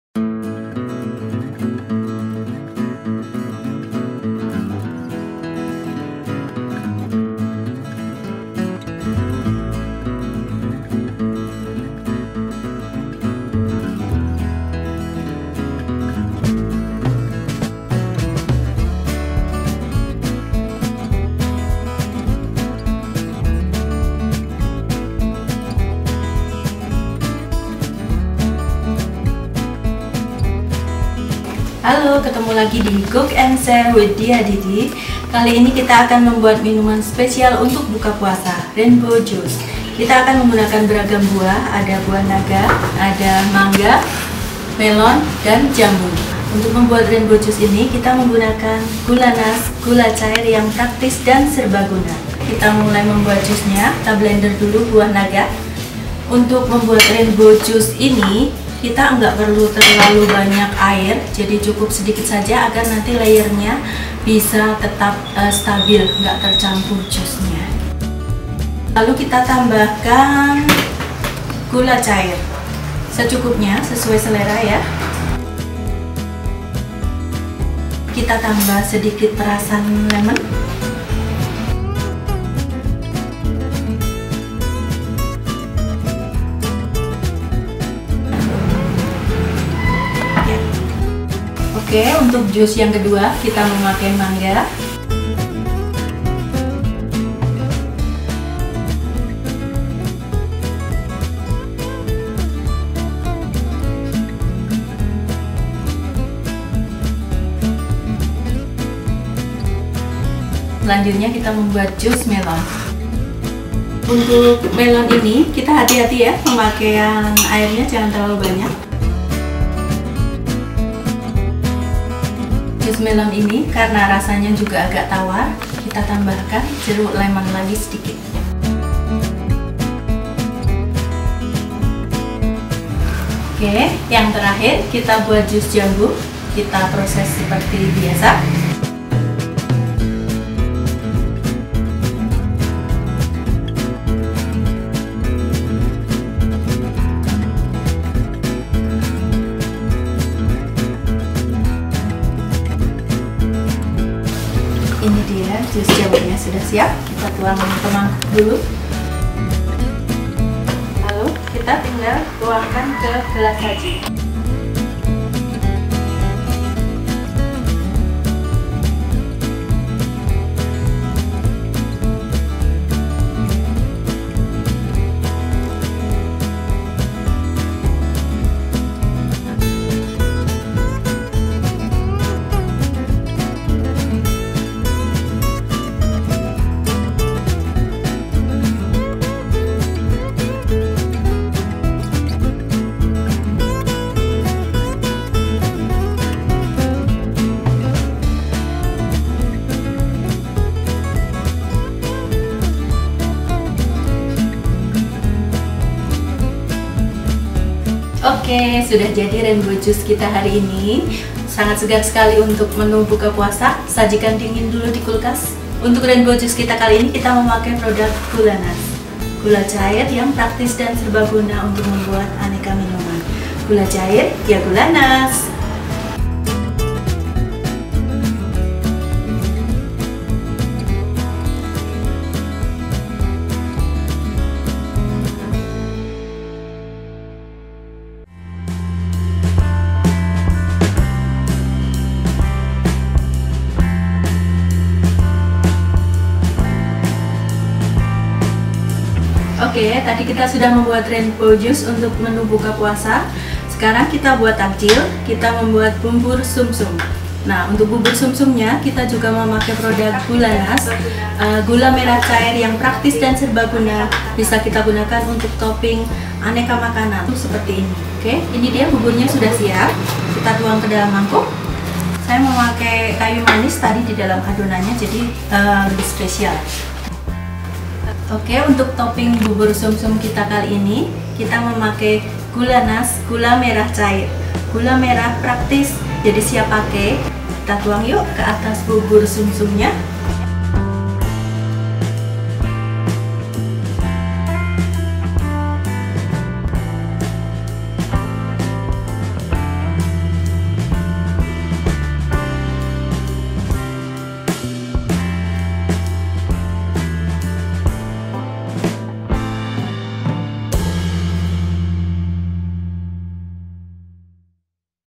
DiahDidi.Com - Rainbow Juice & Bubur Sumsum GULANAS
GULANAS - Gula Cair ? Ya, Gulanas, mempersembahkan resep Rainbow Juice dalam acara Cook & Share with Diah Didi. Please visit DiahDidi.Com